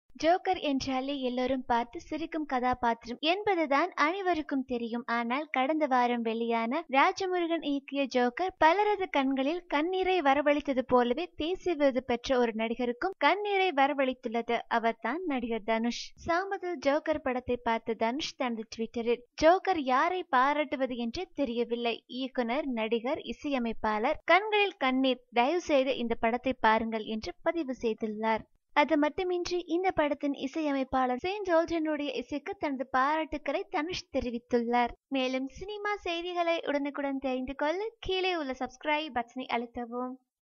Dhanush comments about Joker movie
Dhanush praises Joker movie team! dhanush  appreciates joker Team